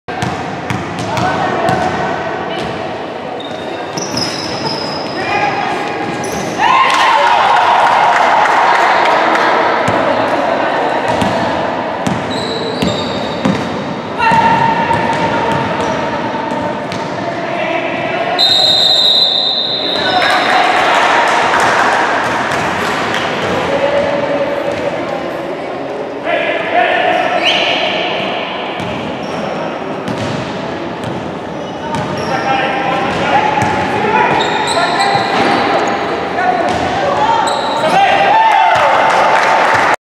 LAS DEMES vs LYNX 19
TORNEO DE MAXIBASQUET 30 + FEMENIL LIMBAVE EN SALTILLO, GIMNASIO JOSÉ DE LAS FUENTES RODRÍGUEZ. ORGANIZA EL PROFE EDUARDO ROJAS LARA.